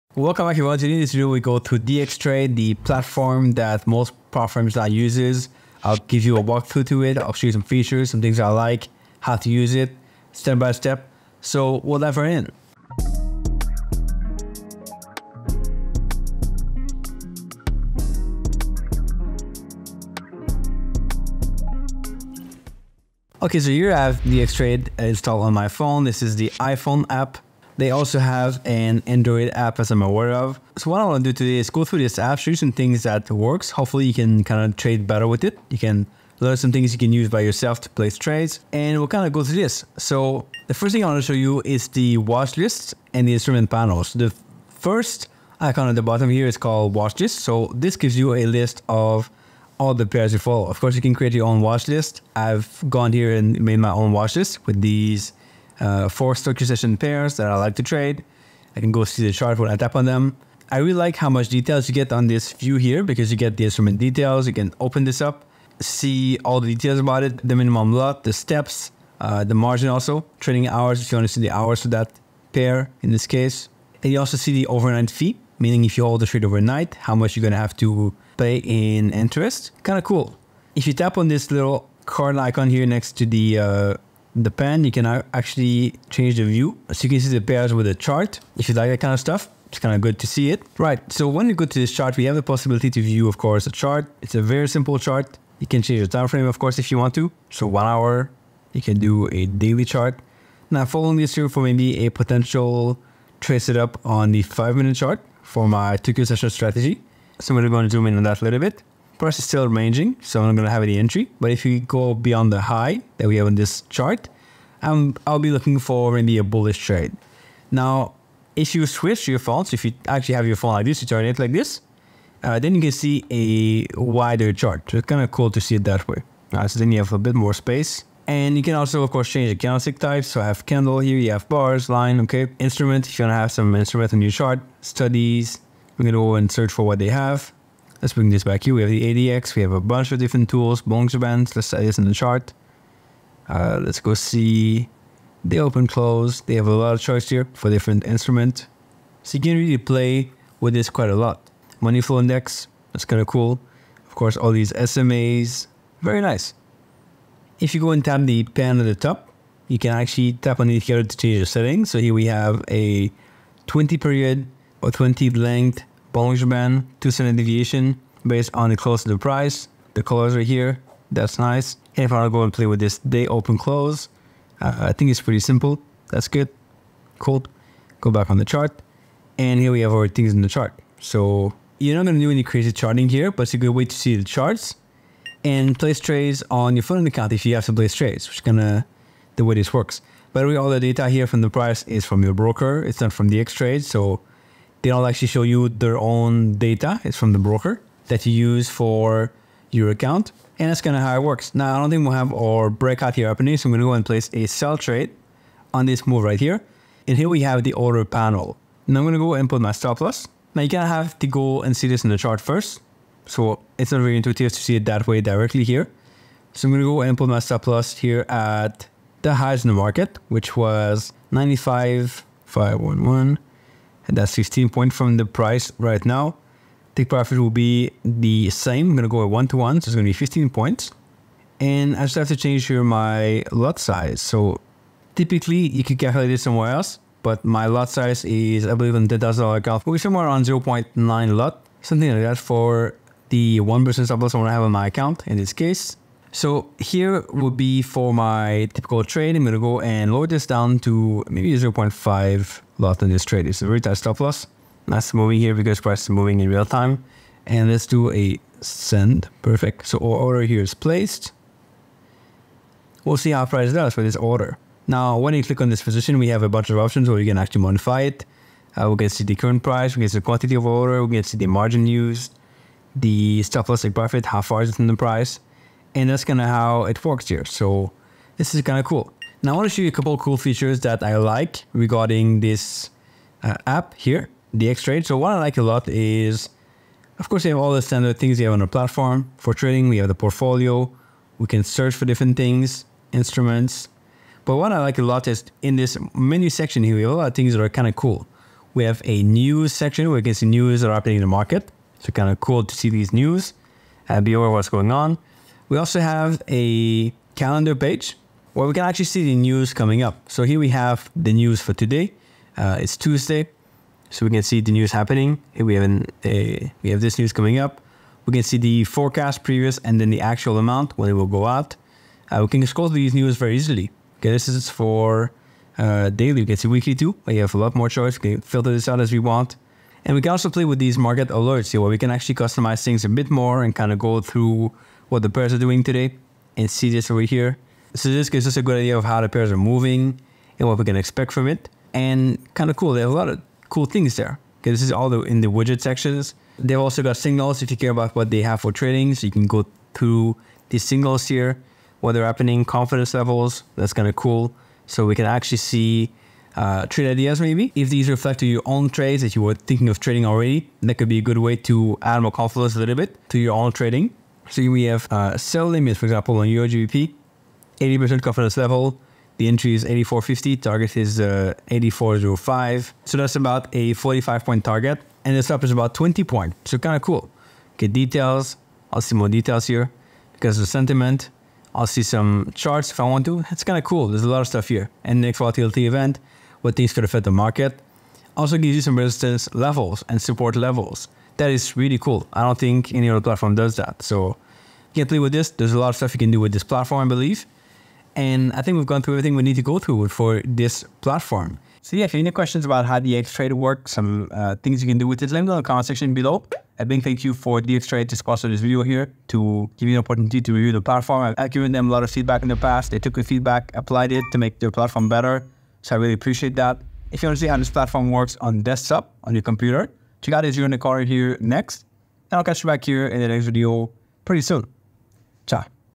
DXtrade Mobile App Tutorial
In this Forex trading video, Etienne shares a full-feature DXtrade mobile app tutorial. Trying to figure out how to use it? Learn everything you need about this prop firm trading app.

🔕 Beware of other fake accounts